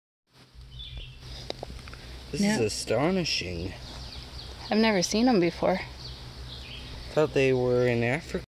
dung beetle Scarabaeinae 01
Dung beetle (Scarabaeinae)

Video by Katie Sandberg
I spotted 3 different pairs of dung beetles while biking Glendalough's bike trails. This was my first time seeing them.